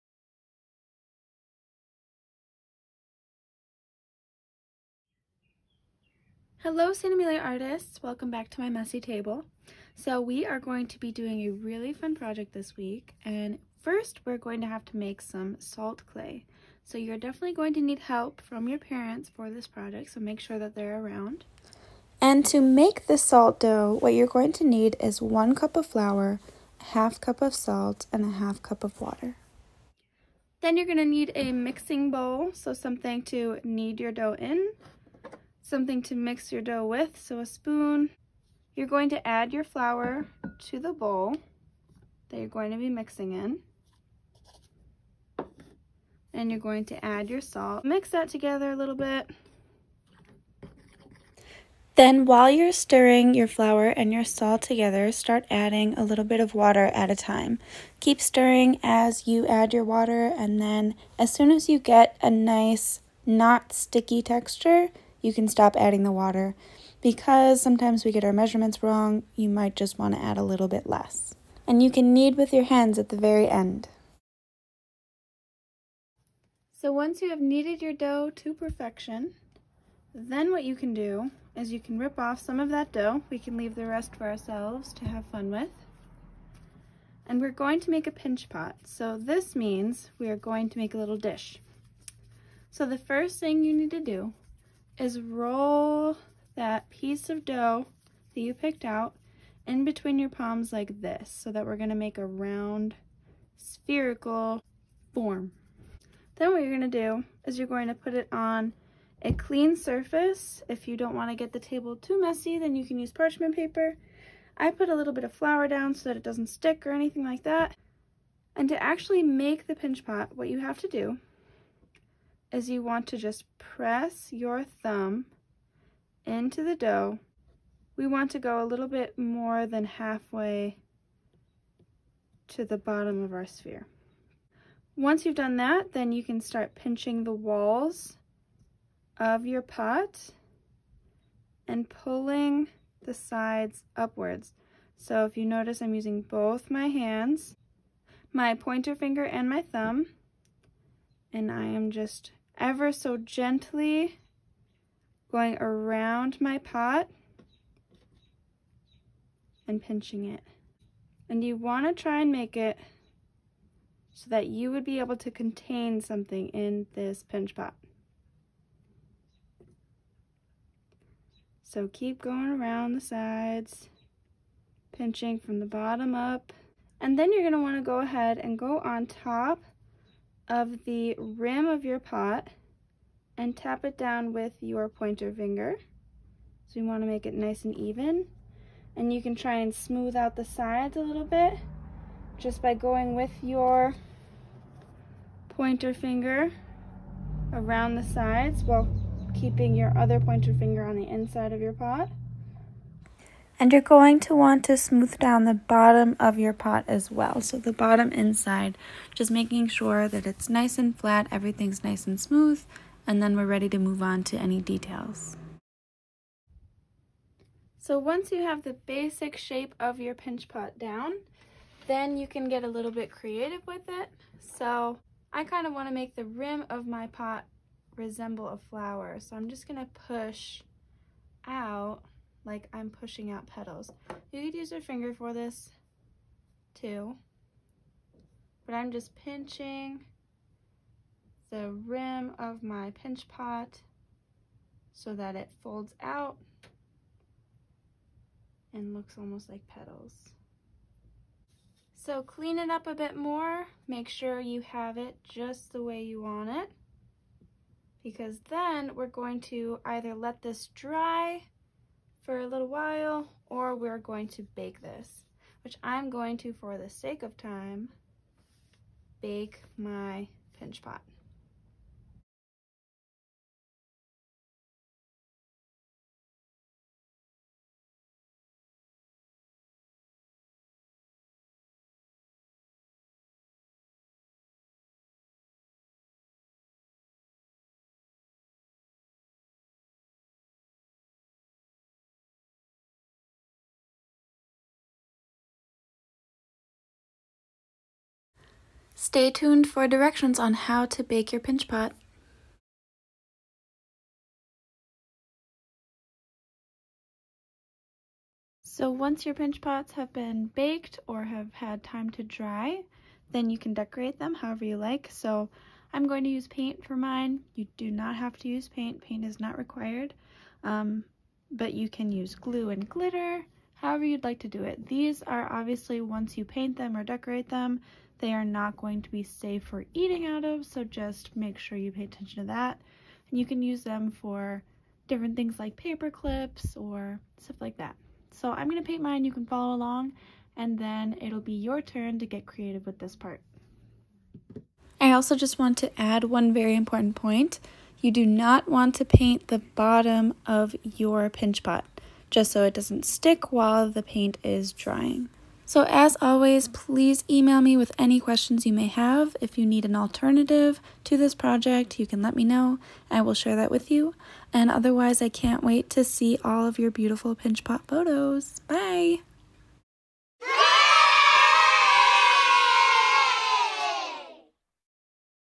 Pinch Pots!
Pk-3rd Grades

Materials:
Flour, Salt, Water
Mixing Bowl
Mixing Spoon
Paint (optional)

Salt dough ingredients:
1 Cup of Flour
1/2 Cup of Salt
1/2 Cup of Water
Stir Salt and Flour together. Incrementally add water and stir. If the mixture is sticky add more flour! Knead to finish off the mixing.

Directions for Baking:
Place your pot on a baking sheet with wax paper
Preheat the oven to 350°
Bake for 15-20 minutes depending on size
Allow time to cool before painting